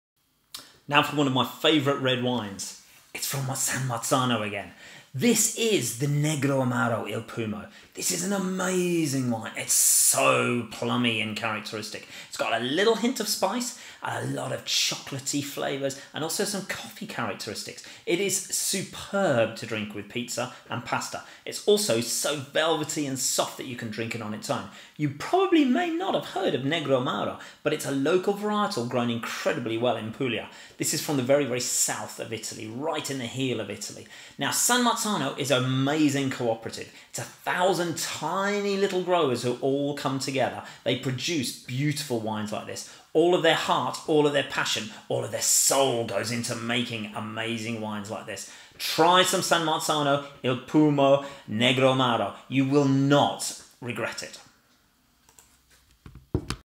"Il Pumo Negroamaro" from San Marzano by @kevinthewineman from Wineman.co.uk
Superb red wine from the San Marzano Cooperative in Puglia.

Follow the Wineman on Facebook for offers, competitions and more wine related stuff that should make you happy